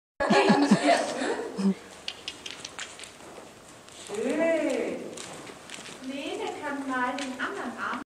Lene und Resi
Das Pferd folgt seitwärts: Körpersprache und Verständnis für gekoppelte Bewegungsabläufe sind nötig